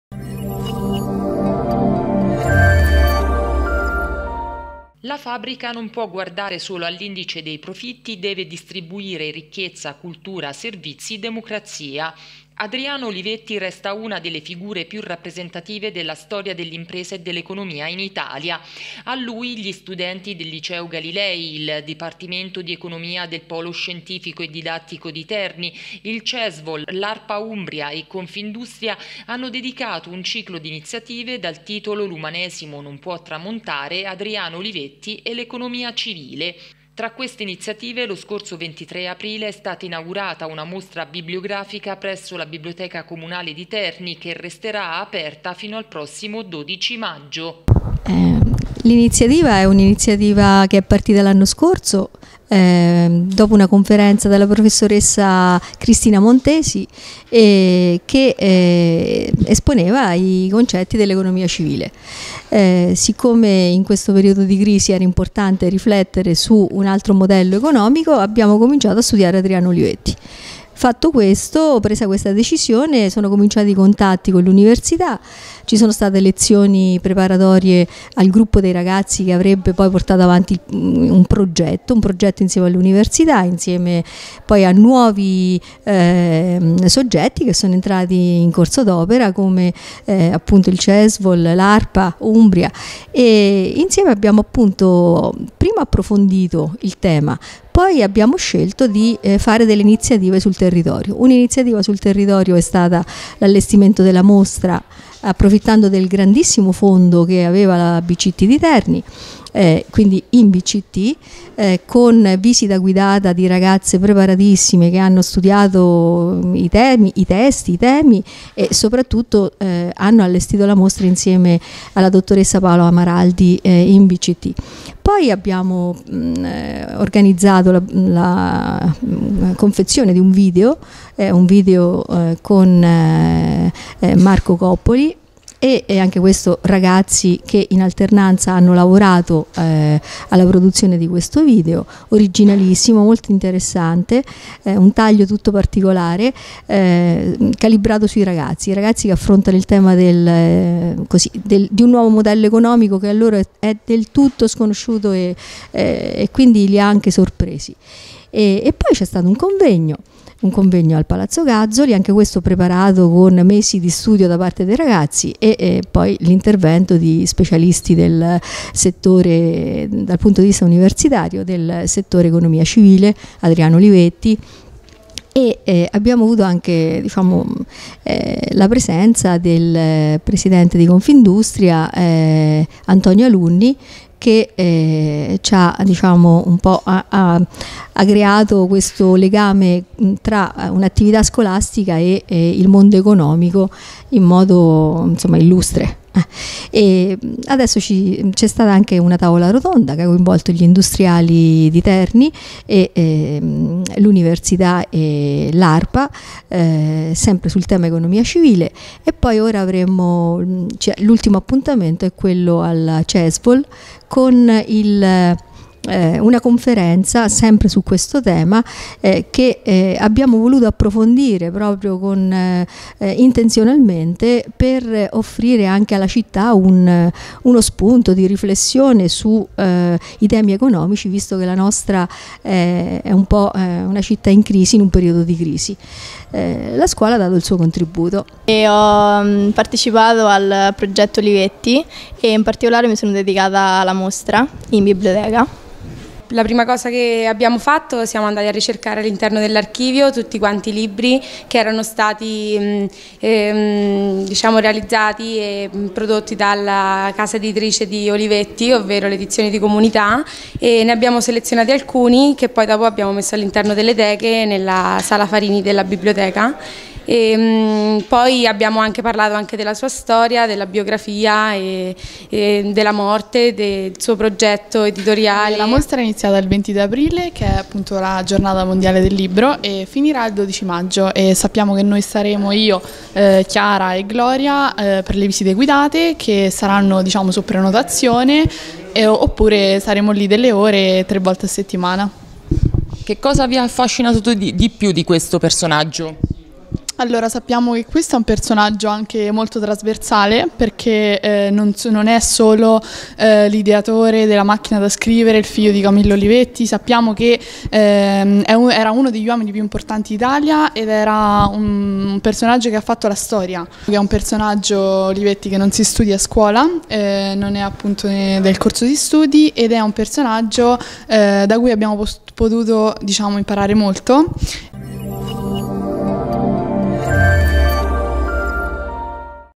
Adriano Olivetti, la mostra degli studenti del Galilei
I ragazzi del liceo Galilei hanno partecipato a un progetto su Adriano Olivetti. Tra le iniziative che hanno curato ci sono una mostra presso la Biblioteca comunale e un video